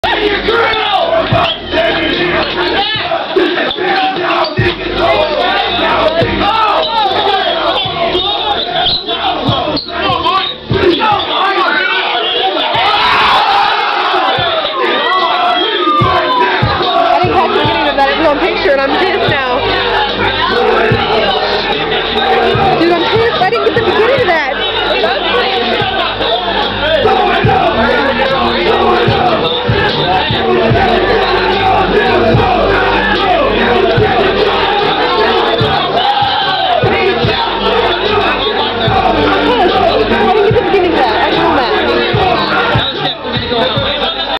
Salem Boxing - 2
Reeder and Meador. (Didn't catch the beginning of it, but it was still a damn good fight. Way to go, boys.)